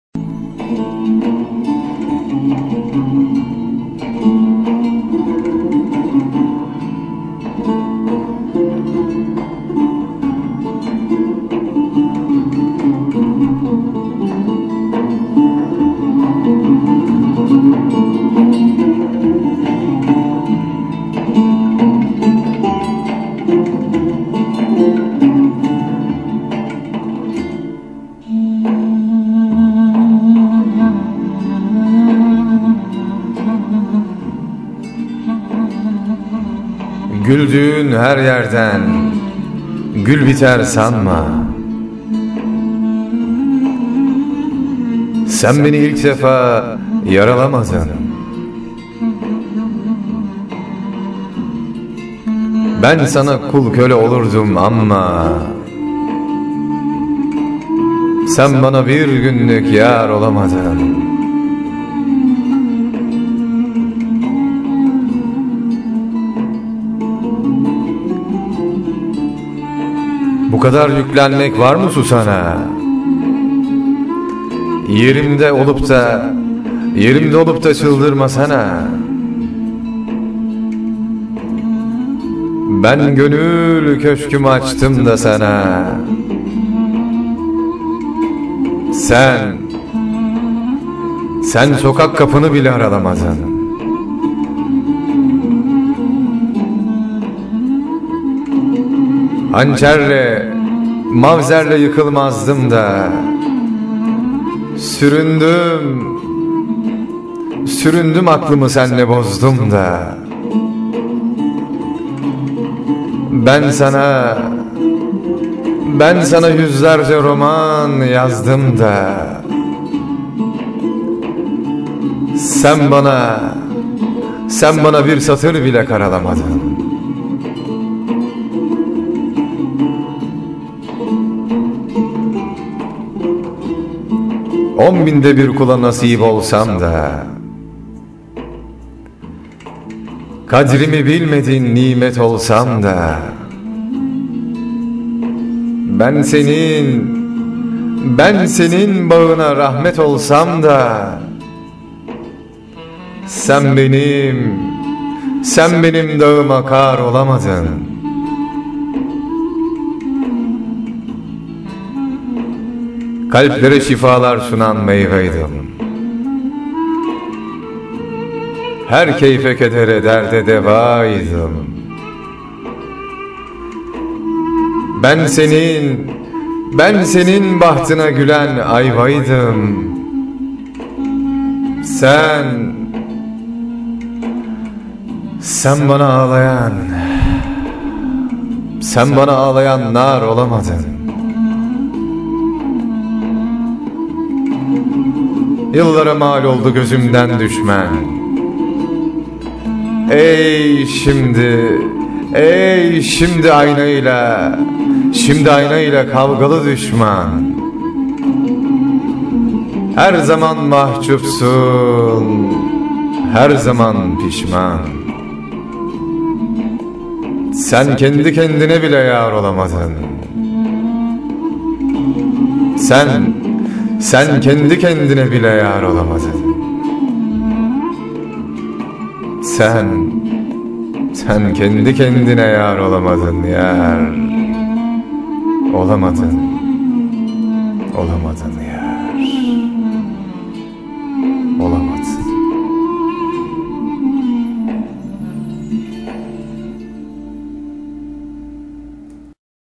Yar olamadın..! Cemal Safì'den bir Aşk şiiri,Serhat Aslanca..
ESER: CEMAL SAFİ

YORUM: SERHAT ASLAN

Yar Olamadın

Vurduğun her yerden gül biter sanma
Sen beni ilk defa yaralamadın
Ben sana kul köle olurdum amma
Sen bana bir günlük yar olamadın

Bu kadar yüklenmek var mı susana
Yerimde olupta çıldırmasana
Ben gönül köşkümü açtmda sana
Sen sokak kapını aralamadın

Hançerle mavzerle yıkılmazdım da
Süründüm aklımı senle bozdum da
Ben sana yüzlerce roman yazdım da
Sen bana bir satır karalamadın

Onbinde bir kula nasip olsam da
Kadrimi bilmedin nimet olsam da
Ben senın bağına rahmet olsam da
Sen benim dağıma kar olamadın

Kalplere şifalar sunan meyvaydım
Her keyfe kedere derde devaydım
Ben senin bahtına gülen ayvaydım
Sen bana ağlayan nar olamadın

Yıllara mal oldu gözümden düşmen
Ey şimdi aynayla kavgalı düşman
Her zaman mahçupsan her zaman pişman
Sen kendi kendine yar olamadın

Cemal Safi

Böyledir Cemal Safi'nin aşk şiirleri..Sürükler,bazen yar kendine bile yar olamaz.Cemal Safi'nin hiç büyütemediği çocuktur içindeki Aşk..